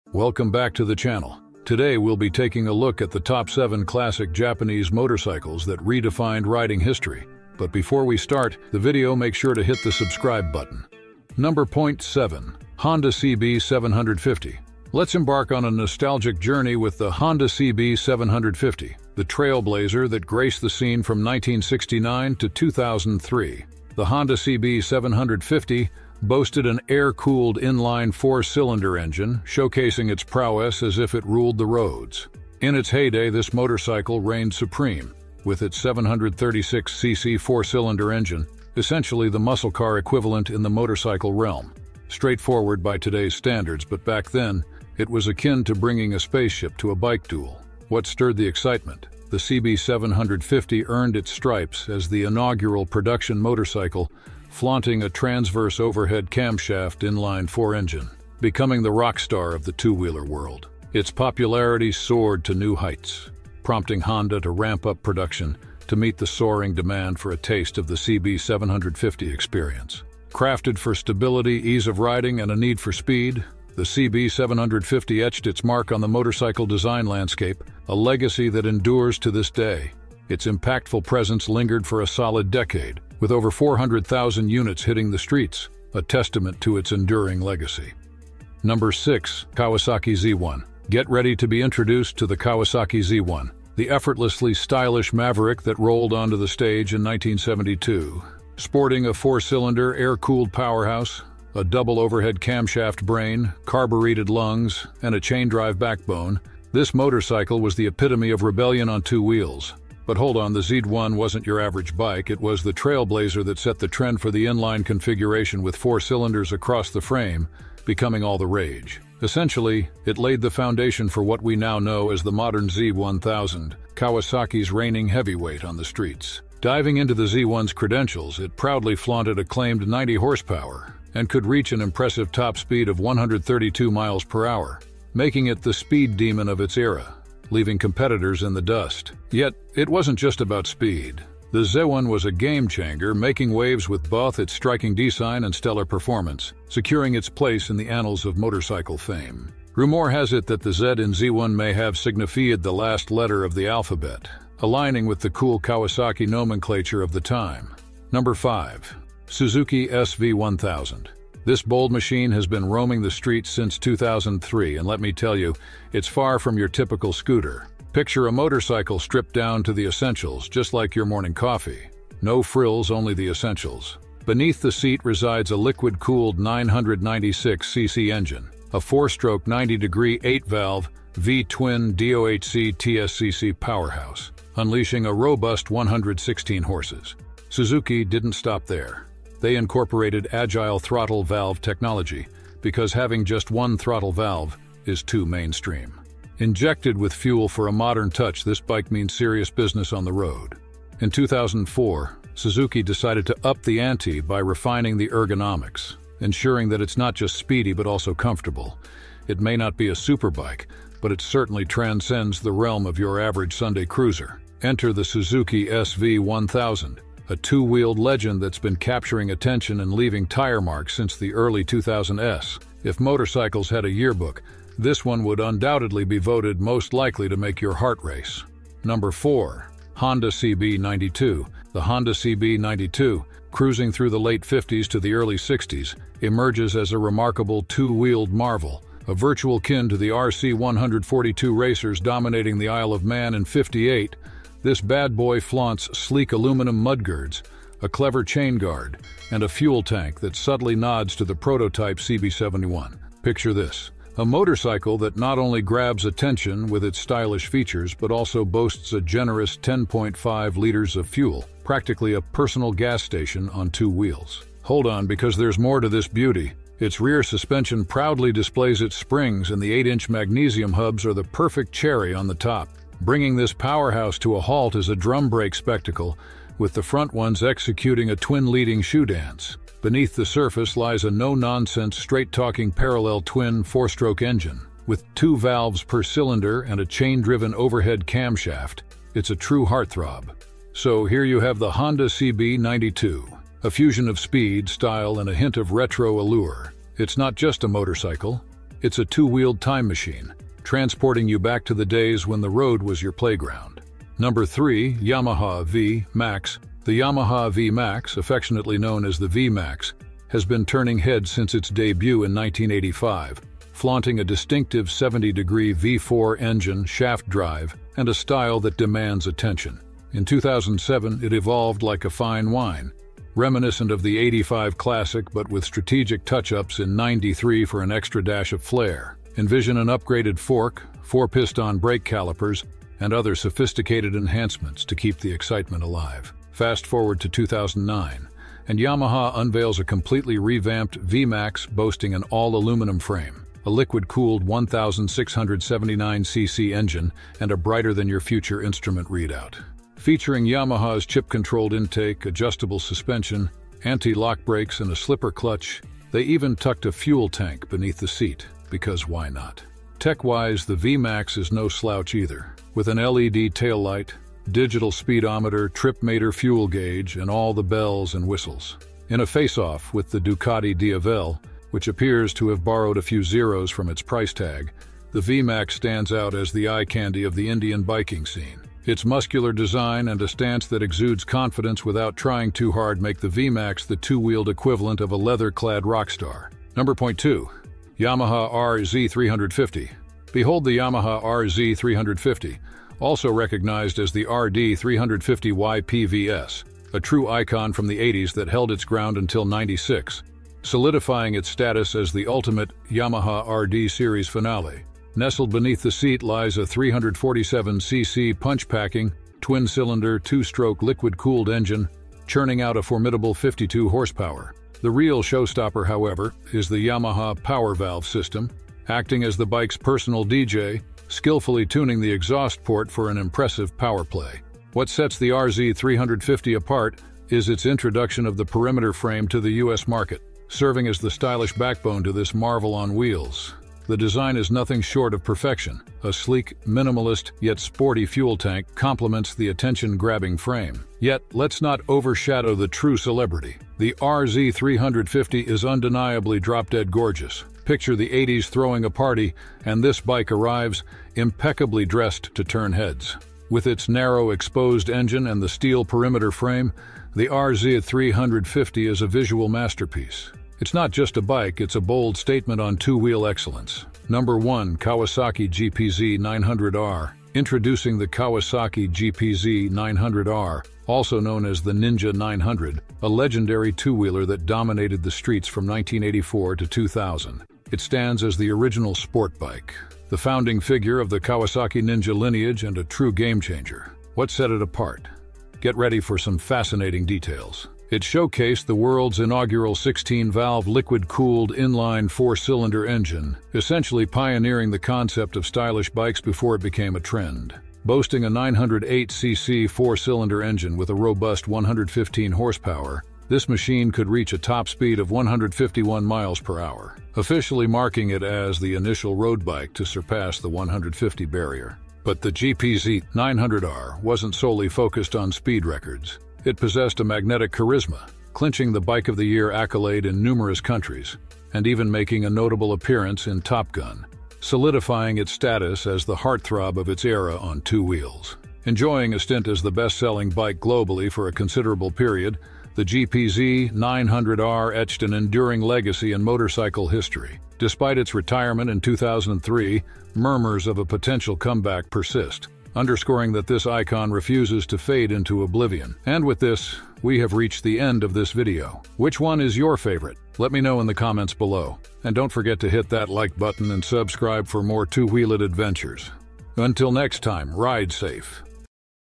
7 Best Classic JAPANESE Motorcycles Ever Made
Welcome back to the channel! Today we'll be taking a look at the Top 7 Classic Japanese Motorcycles That Redefined Riding History .
Honda CB750
 Kawasaki Z1:
 Suzuki SV 1000
 Honda CB92
 Yamaha VMax
 Yamaha RZ350
 Kawasaki GPZ 900R